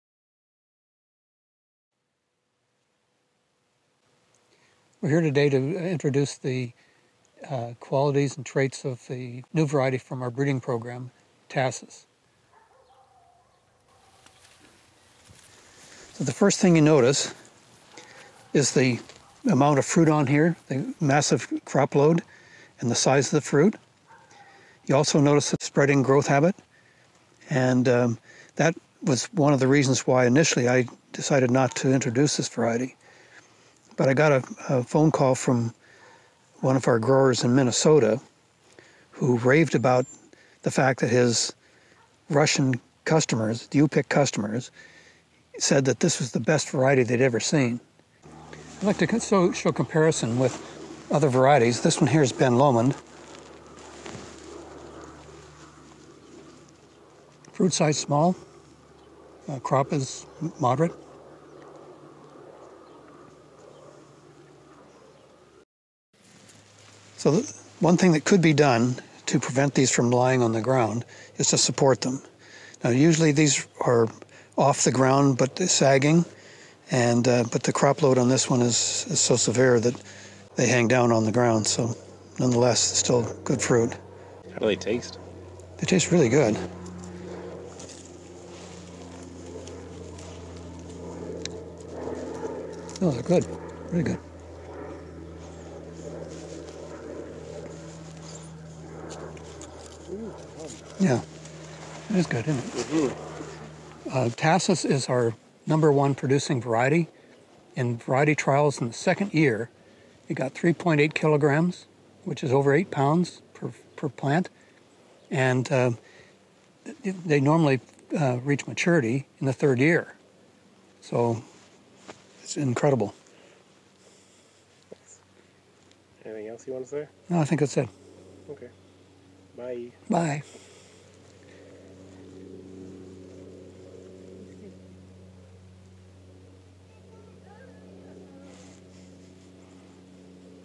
Growing Blackcurrants in North America | Tahsis - Incredible yields, flavor, and growth.
Showcasing the newest blackcurrant variety from the McGinnis Berry Crops breeding program, Tahsis. for ordering and more information.

Tahsis scored the highest yields in replicated trials in coastal British Columbia. Flowers late mid-season, tolerant of late frost, has high levels of resistance to White Pine Blister Rust, resistant to mildew, and has very large fruit (larger than Ben Sarek). Growth habit is spreading which makes it suitable for hand harvest or U-pick, displays the fruit well, but not suitable for machine harvest. Annual pruning is a must.